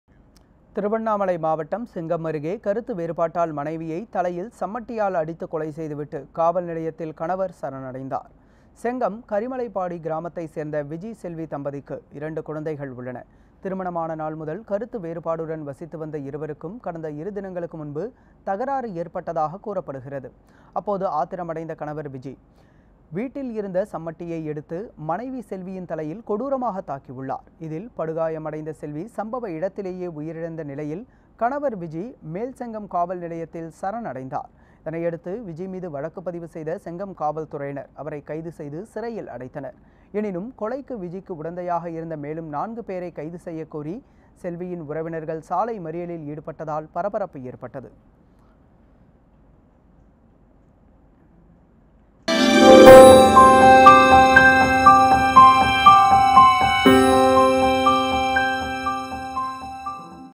தகராறு முற்றியதில் மனைவியை சம்மட்டியால் தாக்கிய கணவர் காவல்நிலையத்தில் சரண் அடைந்தார் | NewsJ
தகராறு முற்றியதில் மனைவியை சம்மட்டியால் தாக்கிய கணவர் விஜி காவல்நிலையத்தில் சரண் அடைந்தார் | NewsJ